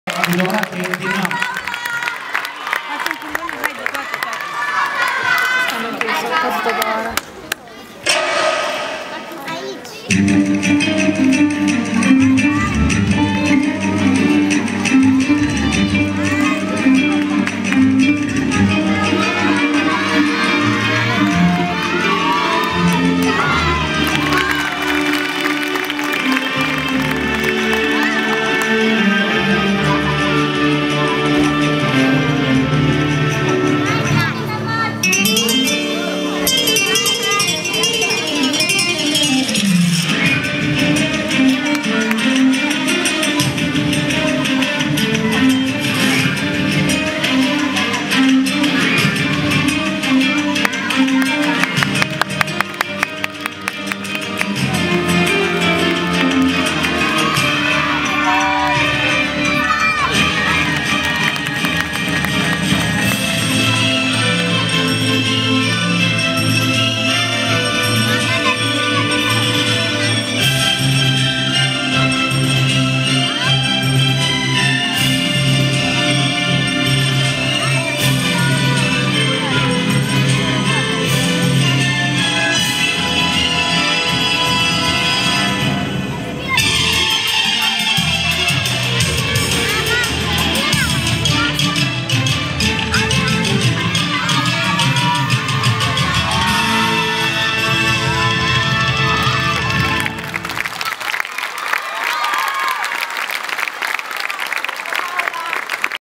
Paula Tudorache - Floor (2014 Romanian Nationals)
Paula Tudorache - Sol (Campionatele Naționale)
Score: 14.100